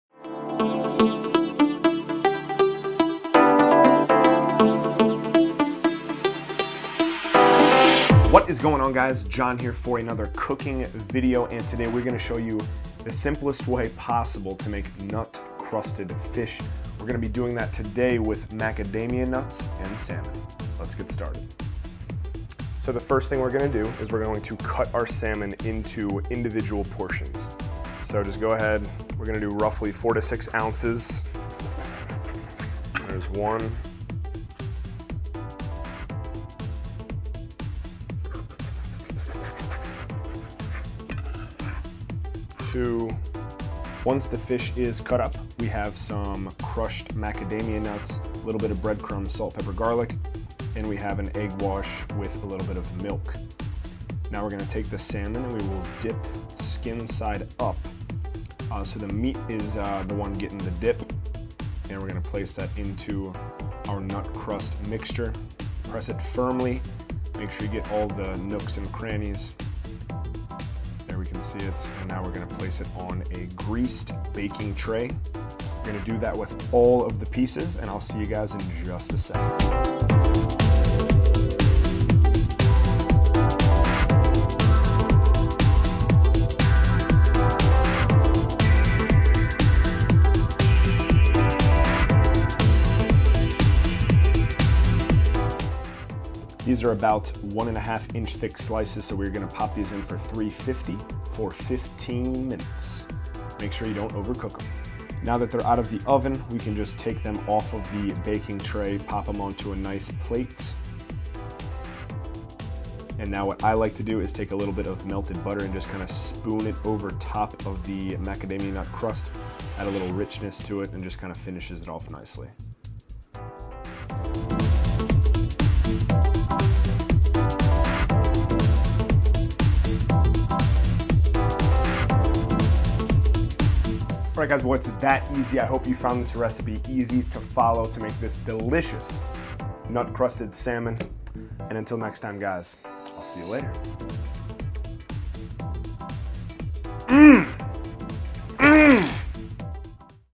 How To Make Nut Crusted Fish!
Today I show you how to make Macadamia Nut Crusted Salmon! This recipe can be interchanged for any type of nut, and any type of fish!

HELP SUPPORT MY FOOD REVIEWS!
YouTube Support: Link on main channel page (top right corner)

SOCIAL MEDIA!